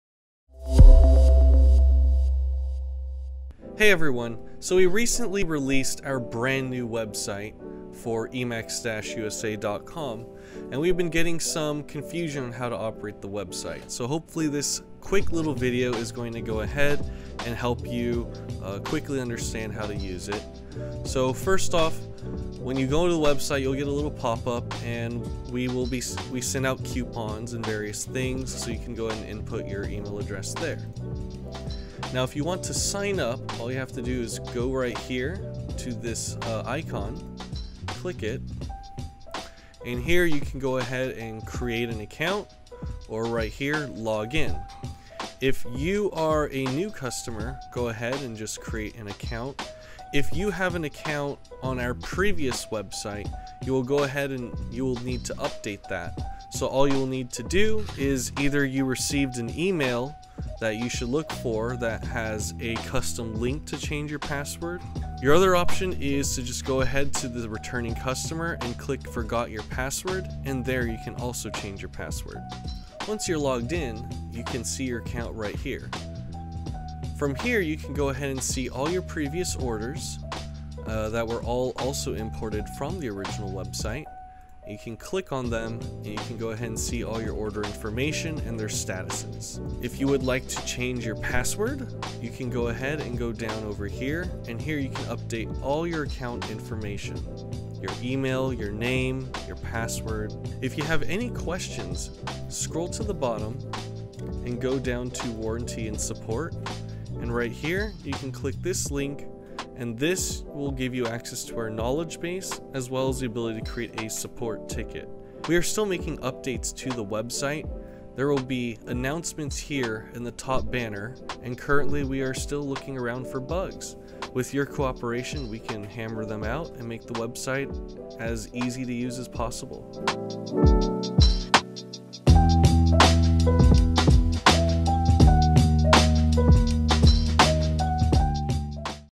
Using the NEW EMAX Website
We recently launched our new website. Here's a brief overview of how to use it, answering some commonly asked questions.